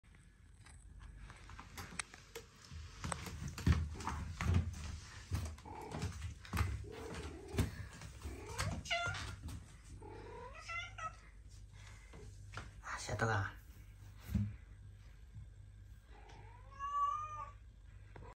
足跡を辿って犯人を追跡してたら、犯人に周回遅れにされた。容疑を否認するウチのニャンコ（The culprit in the footprints is my cat）
タイトルと内容のアンバランスが魅力のチャンネル